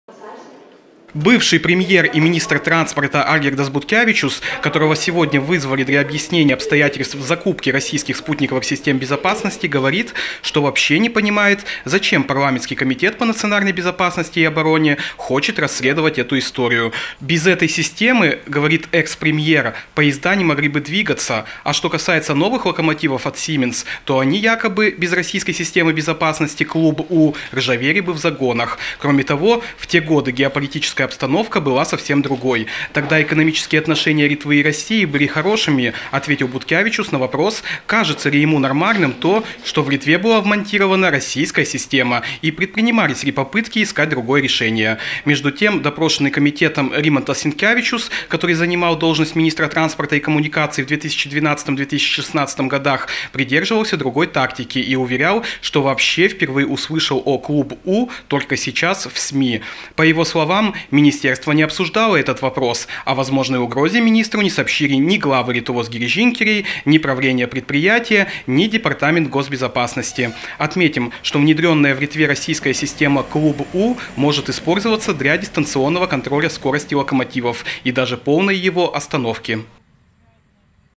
Расследуют обстоятельства закупки российского оборудования
В Сейме проходят разбирательства в связи с закупкой российских систем безопасности, которыми оборудованы поезда Литовских железных дорог. Политики беспокоятся, что тем самым Литва добровольно открыла двери для недружественного соседа, который якобы может теперь дистанционно управлять всеми железнодорожными составами, а значит влиять на безопасность страны.